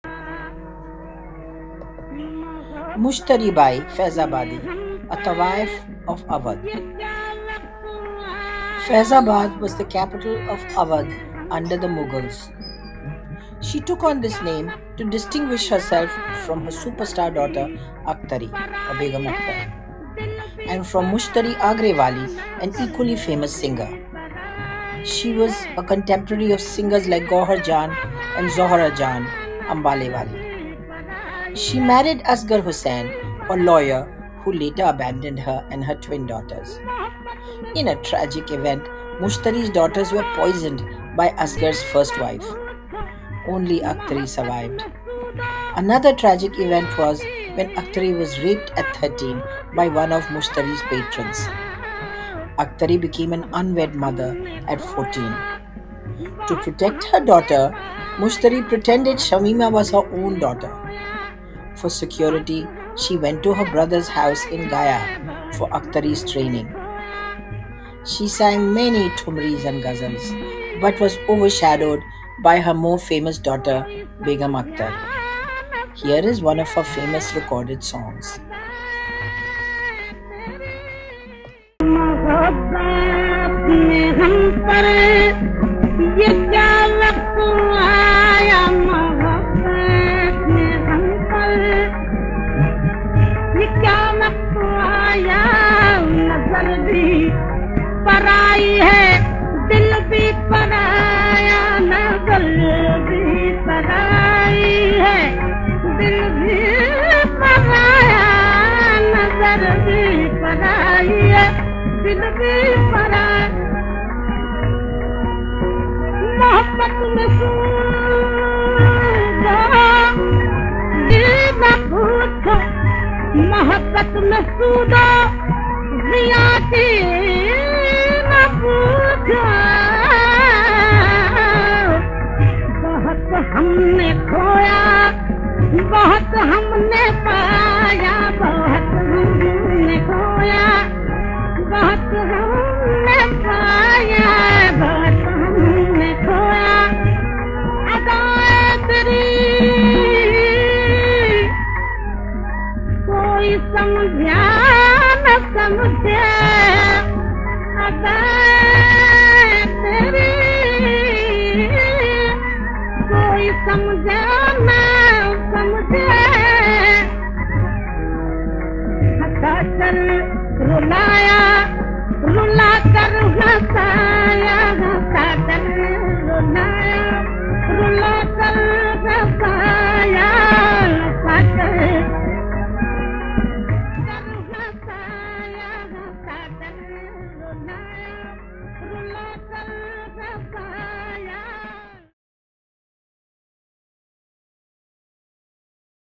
Mushtari Bai Faizabadi-A Tawaif from Awadh
Mushtari Bai, the mother of Begum Akhtar, was a famous recording artist in her own right but allowed herself to be eclipsed by her more famous daughter, Begum Akhtar.